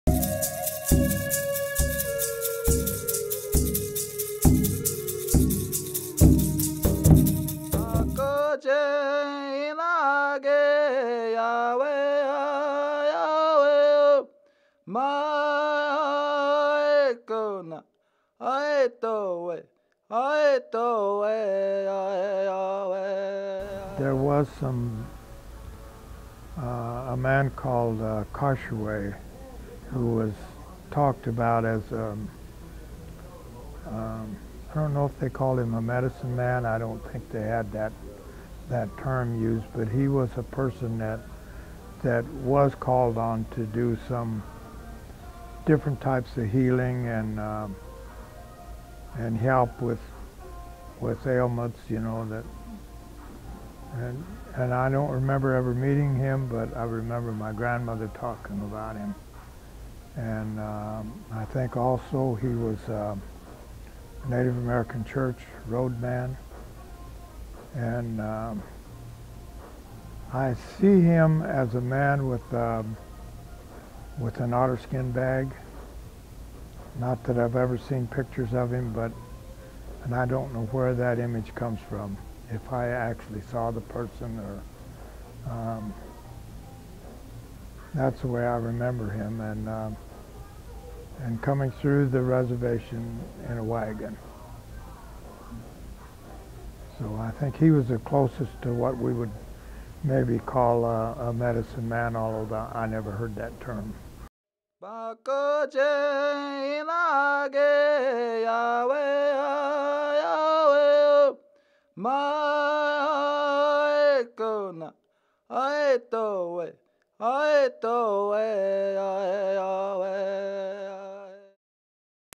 While growing up the Iowa reservation, did you know of any medicine men or healers? with Pete Fee
Spiritual and Physical Health in the Traditions of the Iowa Tribe

By Rebekka Schlichting

Pete Fee is an elder from the Iowa Tribe of Kansas and Nebraska. He grew up in the White Cloud, KS area.